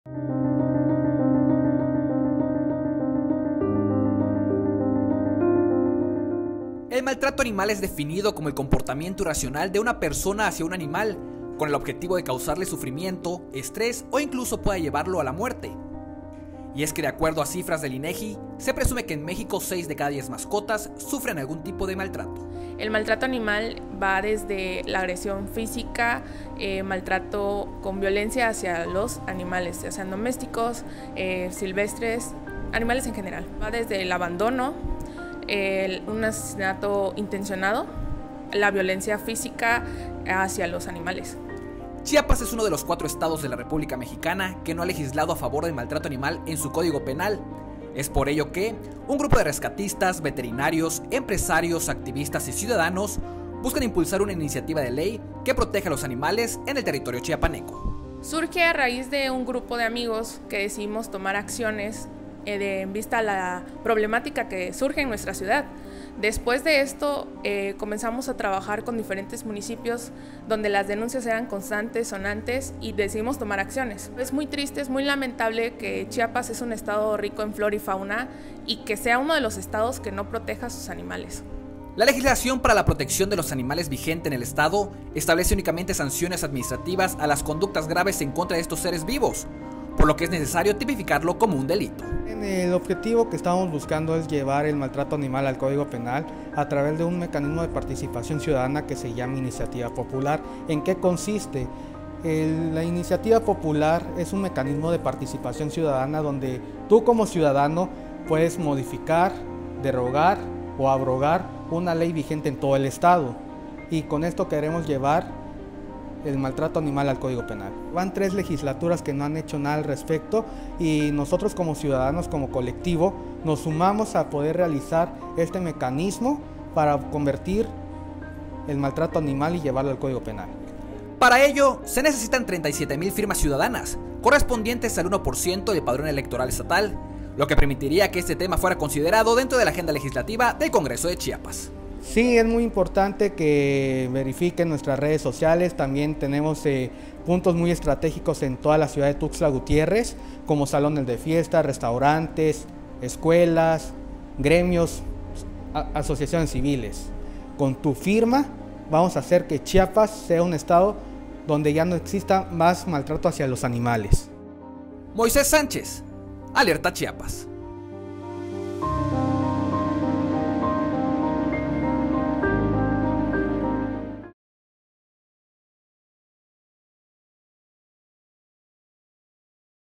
¡Firma contra el maltrato animal en Chiapas!
En Chiapas no es un delito maltratar a los animales; el estado es uno de los cuatro de México que no ha legislado al respecto. Ciudadanos lanzan iniciativa para que diputados legislen al respecto.